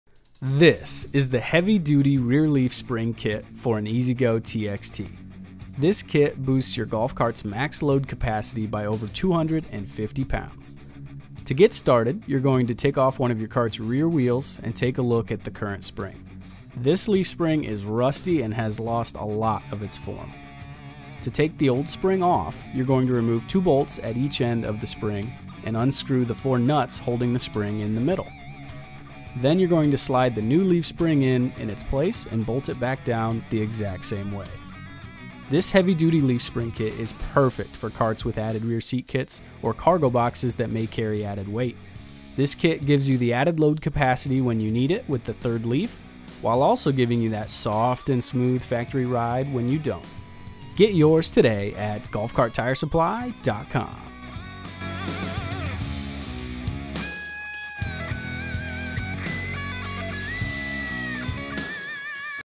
Heavy Duty Rear Leaf Spring Kit for EZGO TXT - Installation!
Beef up the ride of your EZGO TXT Golf Cart with our Heavy Duty Rear Leaf Springs Kit. This rear leaf spring kit is perfect for carts with a rear seat kit or cargo box installed. Increases your cart's overall load capacity +250 lbs while maintaining a soft factory ride under normal rider weight. Under heavy weight (extra passengers, cargo), the extra leafs kick in to help you handle extra payload.

Each kit contains:
(2) x Heavy Duty Leaf Springs
(1) x Full set of Rear Leaf Spring Bushings
(6) x Leaf Spring Bushing Spacers
Uses your existing nuts